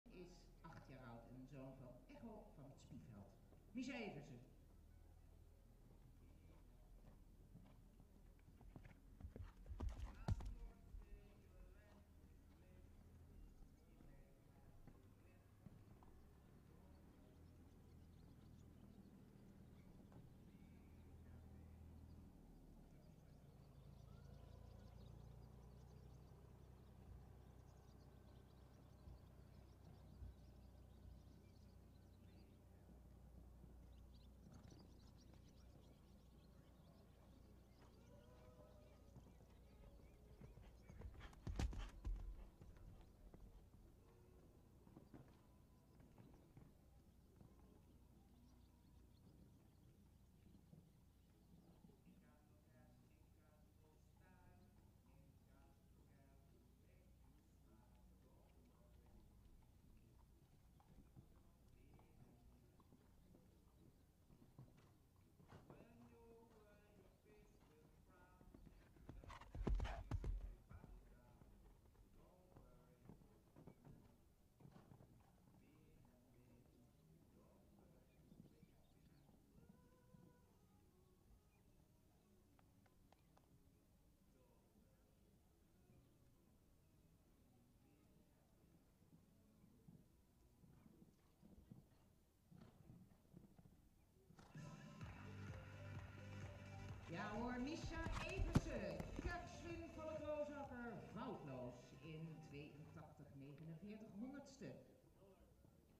Kerswin vh Roosakker in Delft by Echo vh Spieveld born 2010
Kerswin vh Roosakker in Delft by Echo vh Spieveld born 2010 ridden by Mischa Everse for Stal Everse